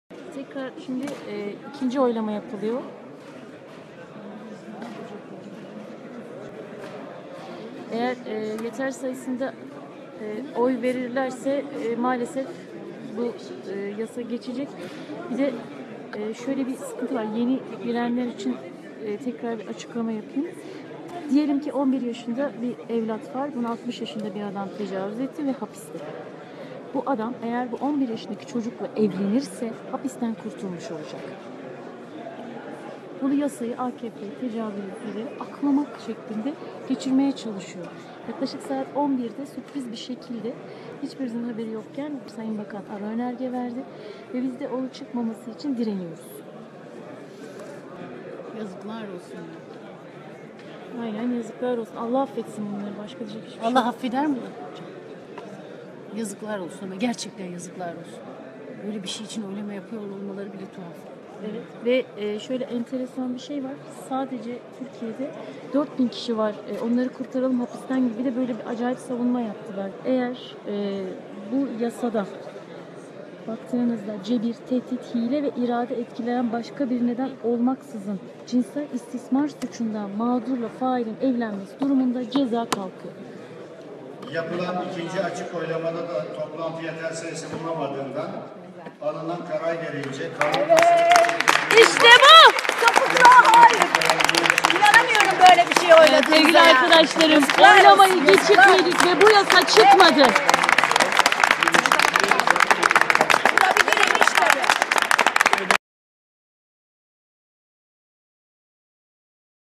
Mücadelemiz Devam Edecek!
Denizli Milletvekilimiz Melike Basmacı, skandal önergeyi Meclis'ten geçirmeye çalışan AKP grubunu görüntüledi. Tecavüzcüleri koruyan, çocuklarımızı felakete sürükleyecek bu tasarının Meclis'ten geçmesine izin vermeyeceğiz!